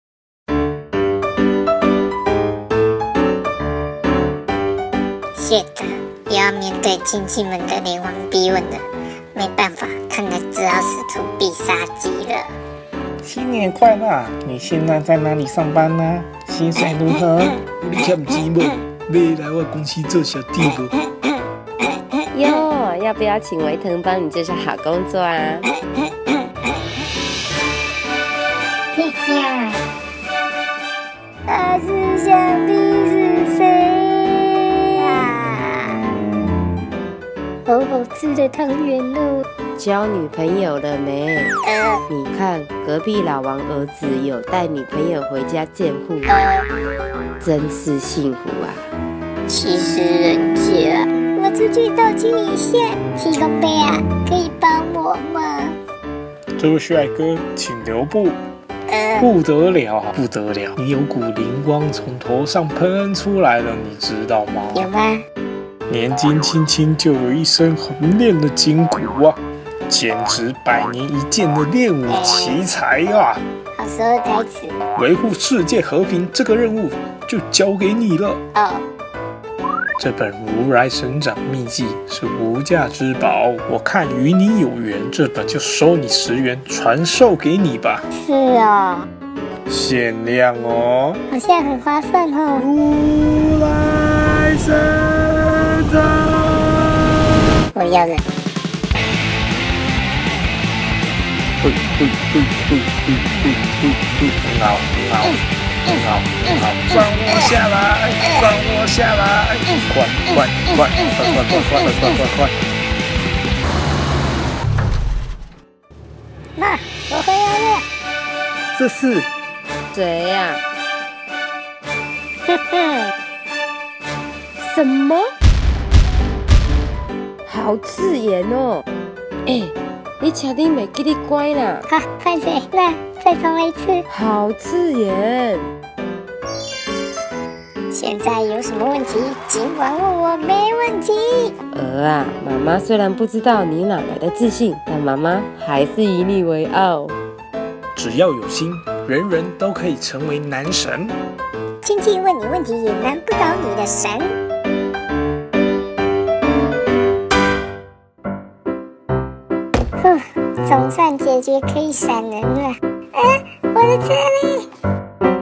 【國泰人壽X毛毛蟲】男神養成篇
你是否也和毛毛蟲一樣總害怕親戚們潮水般的「溫暖」關心呢？看看毛毛蟲如何背負眾人期待躍身成為新一代男神？

國泰人壽AG2.0「不e樣」業務菁英養成計劃
早一步開始，早點成為「人生勝利組」！